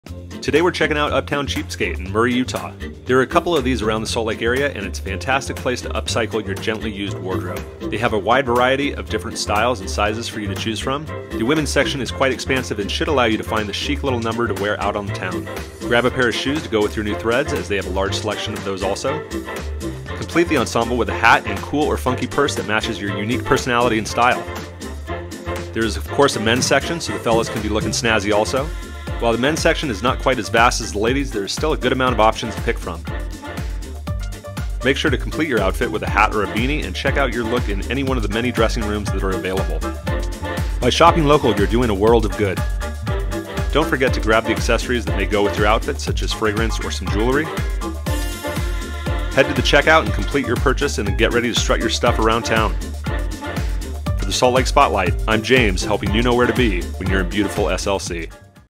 Uptown Cheapskate (Murray, UT)
Uptown Cheapskate is a hip and chic way to upgrade your wardrobe. Bring your gently used clothing or shoes in and get cash you can use towards getting some cool new threads!

Uptown Cheapskate
264 E. Winchester St.